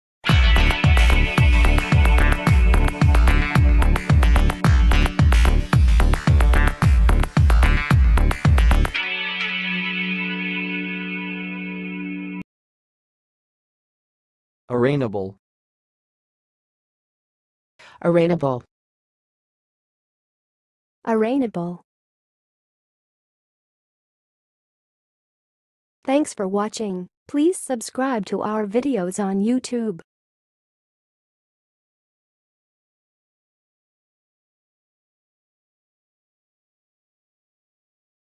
How to say "arraignable"! (High Quality Voices)
Watch in this video how to say and pronounce "arraignable"!